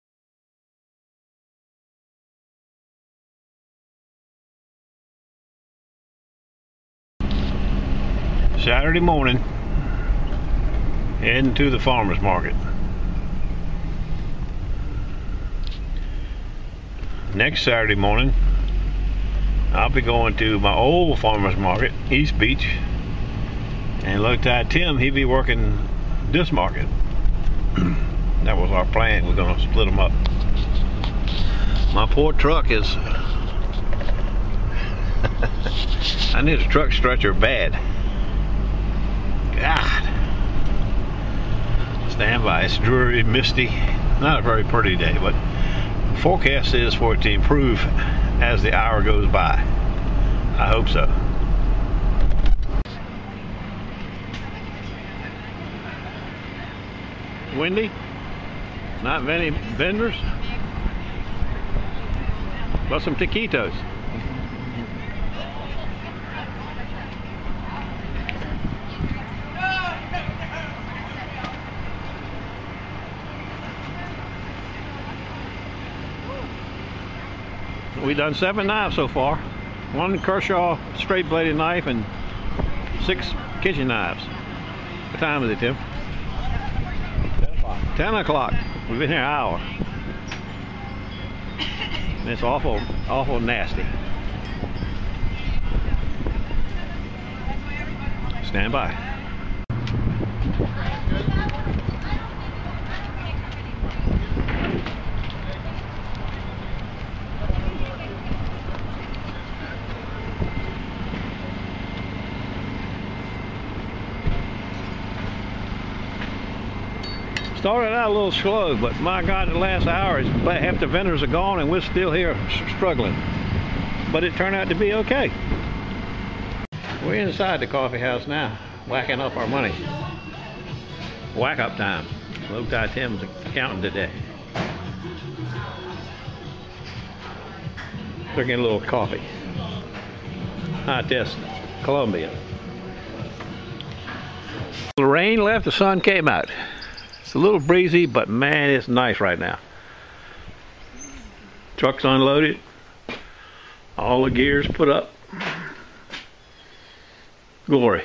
APRIL 30TH - Tim and I work the Shore Drive Farmers Market today | Belt disc sander
APRIL 30TH - Tim and I work the Shore Drive Farmers Market today